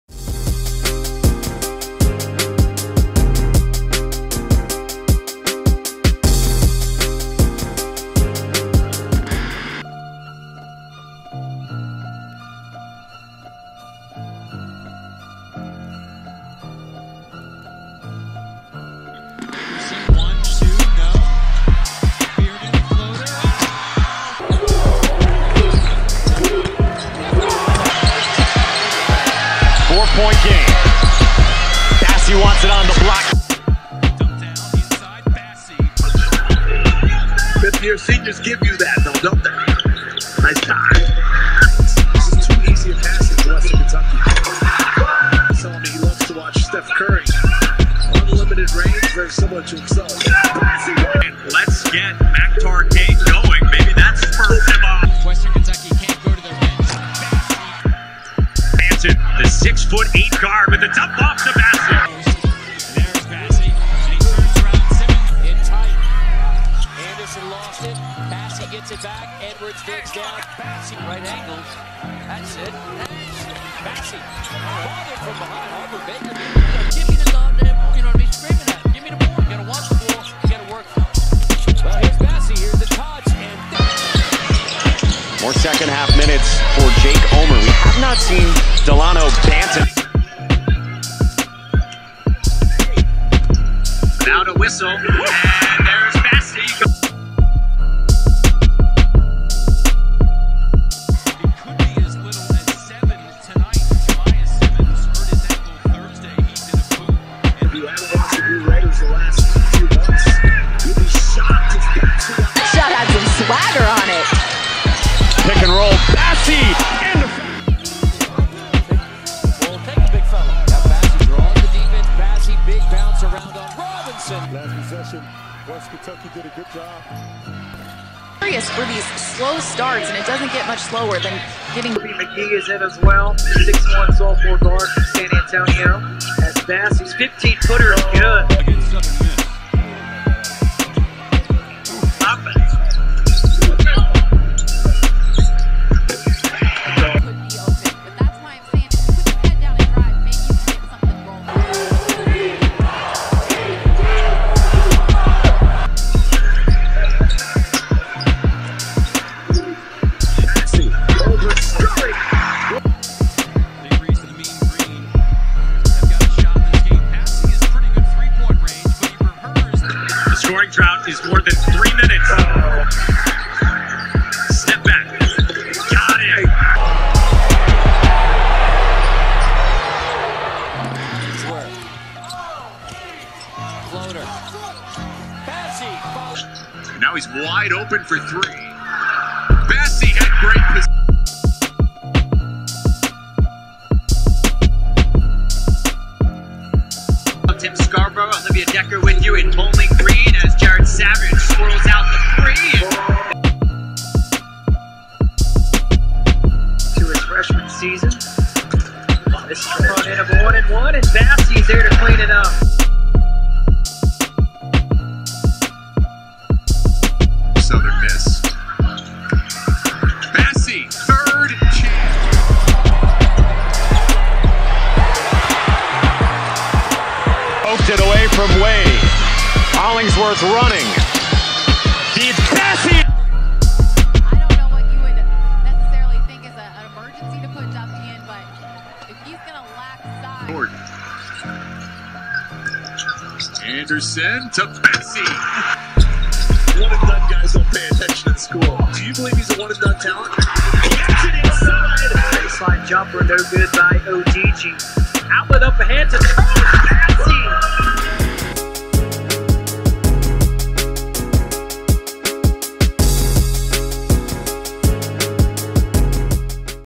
2019 NBA Draft Junkies Profile | Charles Bassey - Offensive Strengths
STRENGTHS
-Great size, strength and wingspan
-Good athlete and has the ability to run the floor in transition
-Strong rebounder
-Good free throw shooter
-Shows potential to be able to extend his range
-Shot blocking/rim protecting
-Efficient around the rim

Instrumentals by Jimmy Del Negro